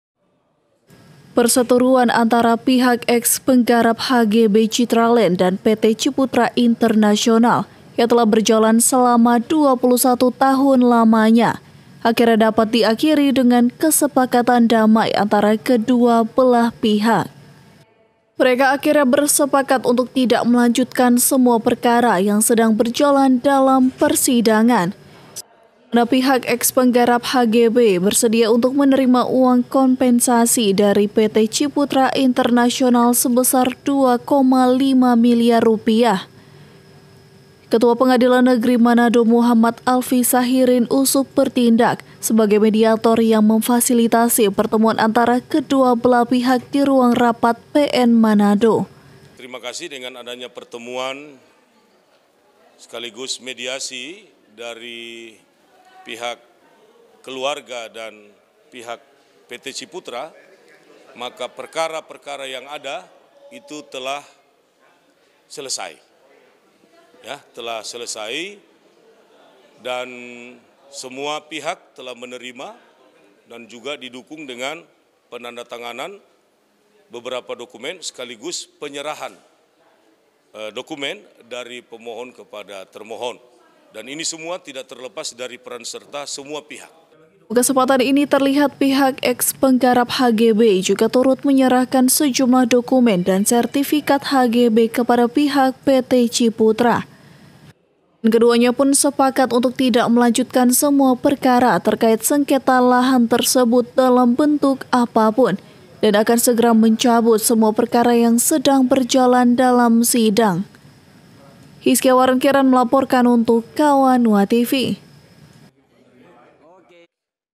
Pengadilan Negeri Manado Mediasi Sengketa Lahan
Warga eks penggarap lahan hal guna bangunan (HGB) dan PT Ciputra Internasional terkait sengketa lahan akhirnya berakhir damai setelah 21 tahun.